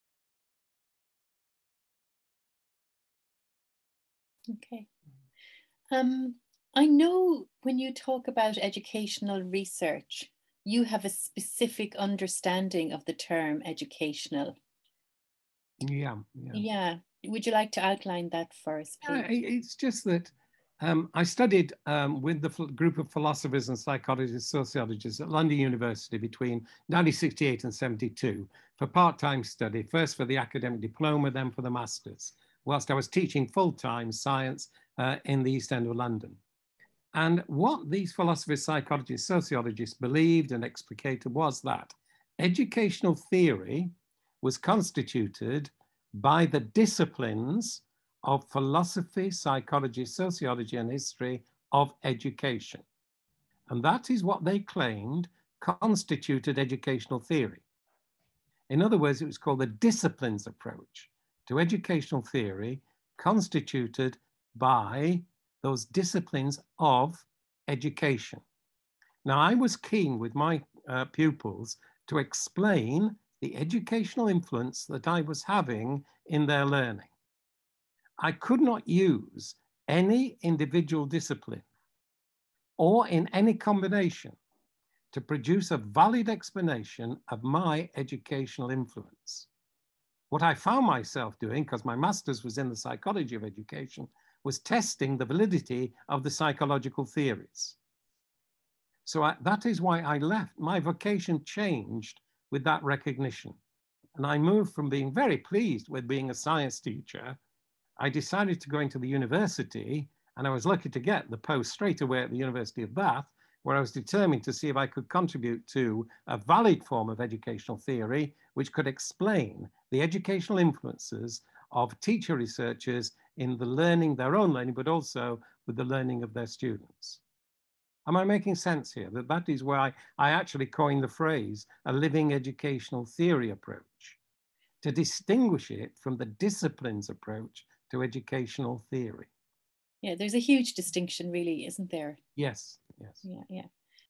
What do you mean by 'educational'?
NEARI convenors, Bernie Sullivan and Máirín Glenn in conversation with Prof. Jack Whitehead, the originator of Living Educational Theory research.